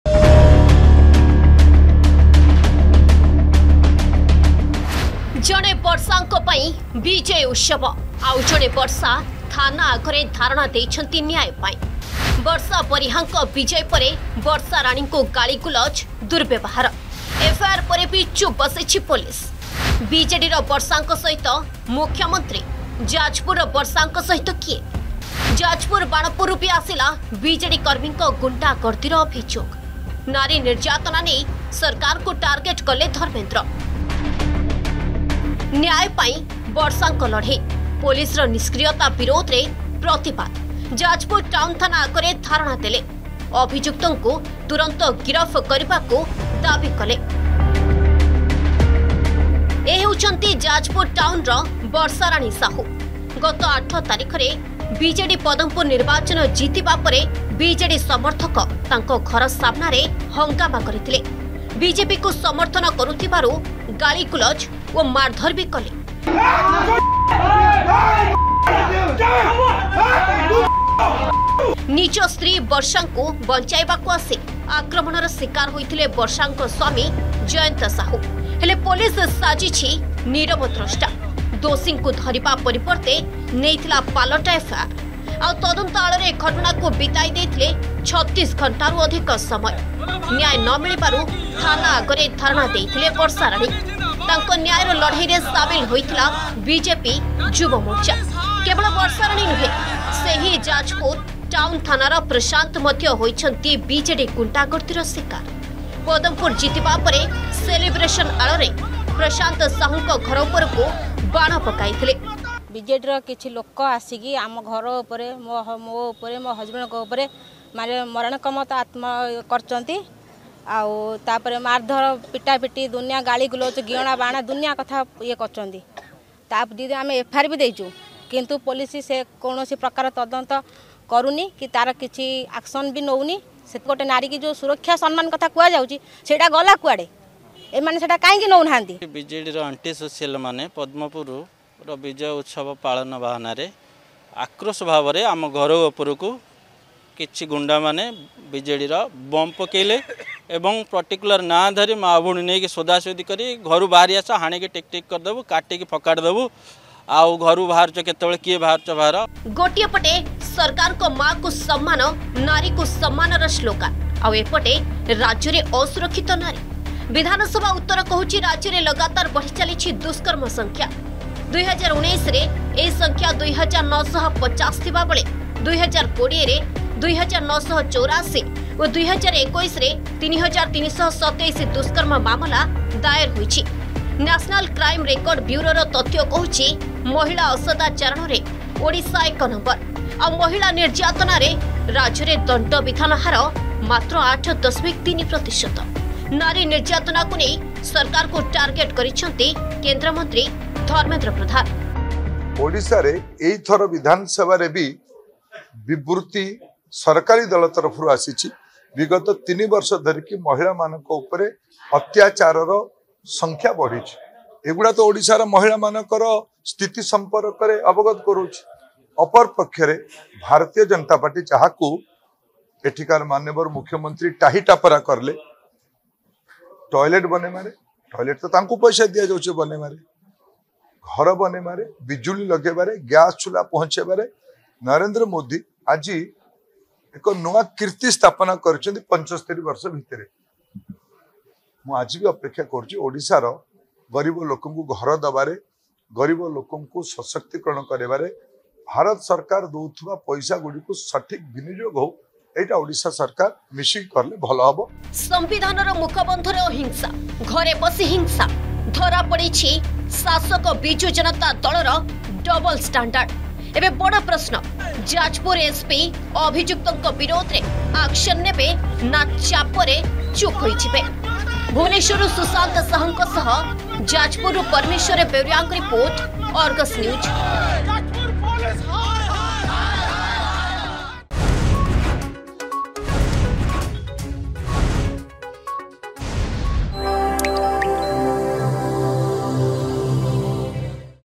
ବିଜେଡିର ବର୍ଷାଙ୍କ ସହିତ ମୁଖ୍ୟମନ୍ତ୍ରୀ, ଯାଜପୁର ବର୍ଷାଙ୍କ ସହିତ କିଏ
ଯାଜପୁର ବାଣପୁରରେ ବର୍ଷାରାଣୀଙ୍କୁ ବିଜେଡି ନେତାଙ୍କ ଦୁର୍ବ୍ୟବହାର ନେଇ FIR ପରେ ବି ଚୁପ୍ ପୋଲିସ। ନାରୀ ନିର୍ଯାତନା ନେଇ ସରକାରଙ୍କ ଉପରେ ବର୍ଷିଲେ କେନ୍ଦ୍ରମନ୍ତ୍ରୀ ଧର୍ମେନ୍ଦ୍ର ପ୍ରଧାନ…।